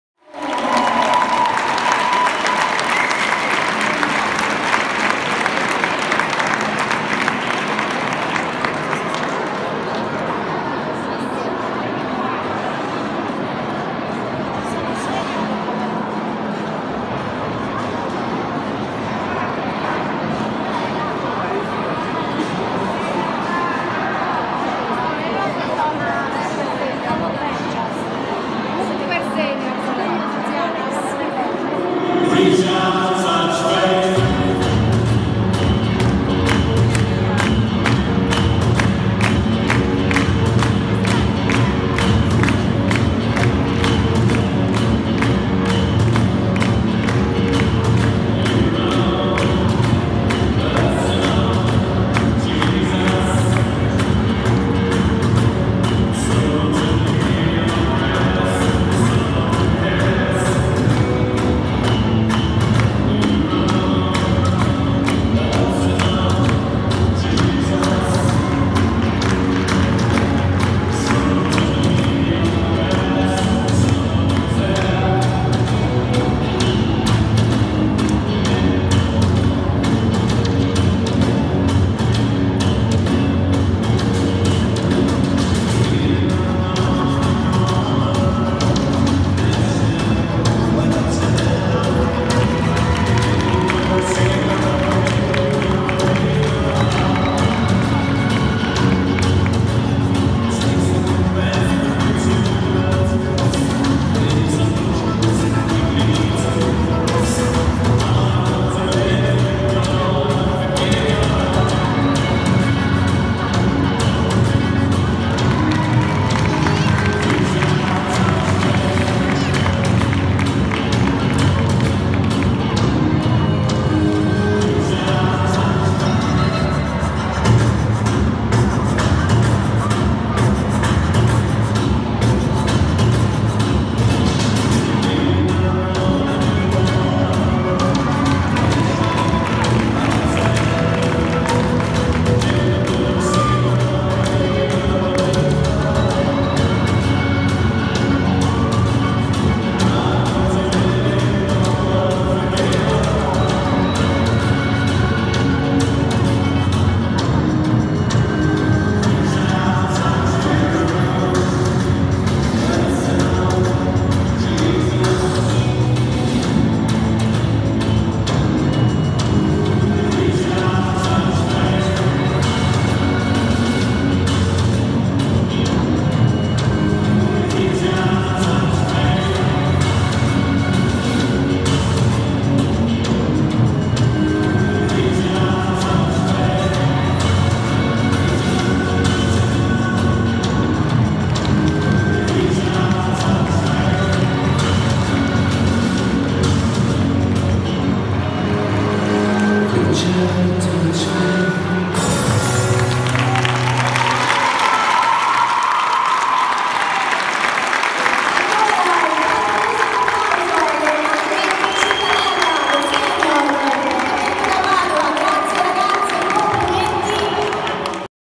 10° Gran Galà delle Majorettes - Cittadella Senior - 15 Ottobre 2017
Esibizione delle Majorettes di Anguillara Sabazia
Palazzetto dello sport di Monterotondo 15 Ottobre 2017
A cura di Roberta Ruggeri